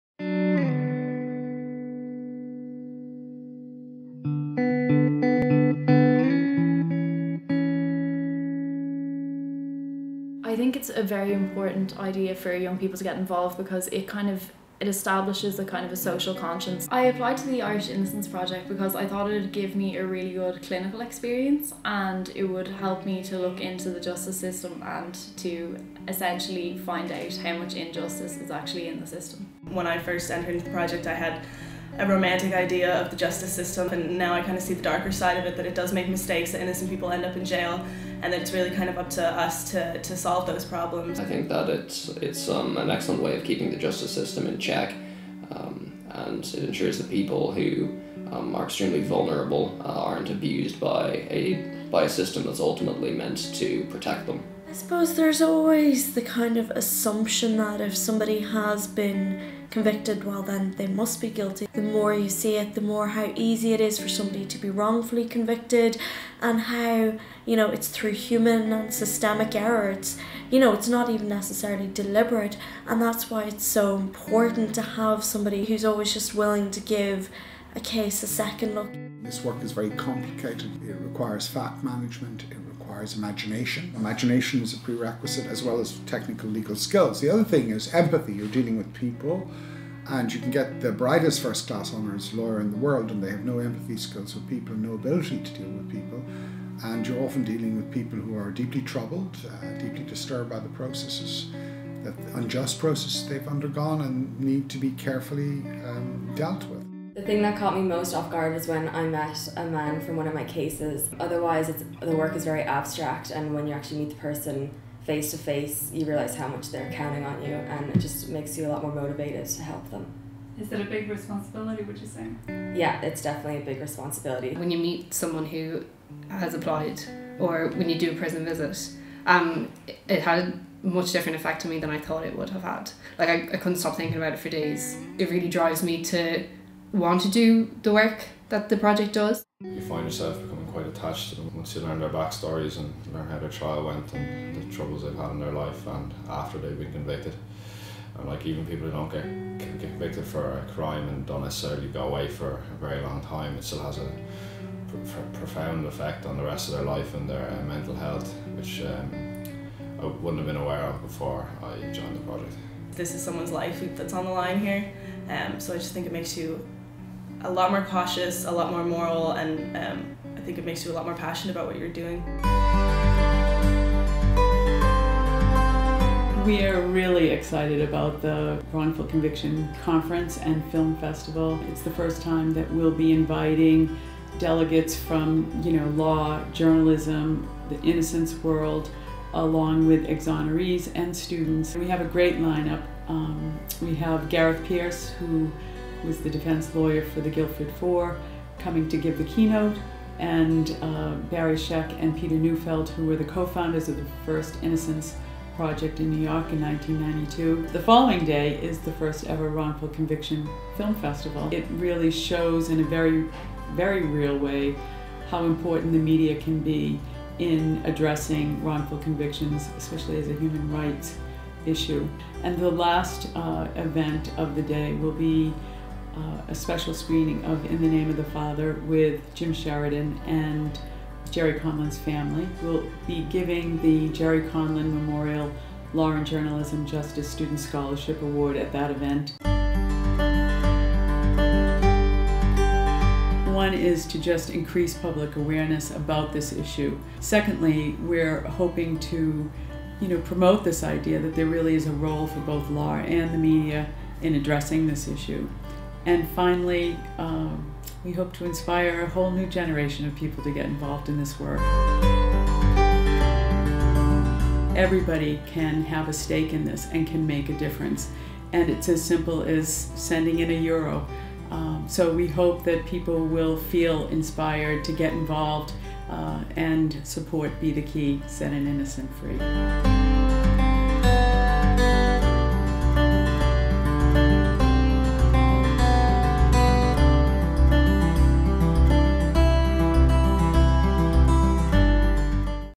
Be the Key: Set an Innocent Free - Meet the caseworkers
The Irish Innocence Project is launching a crowdfunding campaign to subsidies students attending the Irish Innocence Project International Conference on wrongful convictions, human rights and student experience, and the Wrongful Conviction Film Festival in Dublin on June 26-27, 2015. The Irish Innocence Project is working to exonerate people who has been wrongfully convicted. The project currently have 21 student caseworkers and 7 supervising lawyers working on 25 cases in Ireland. The campaign will start March 31 and run for a month.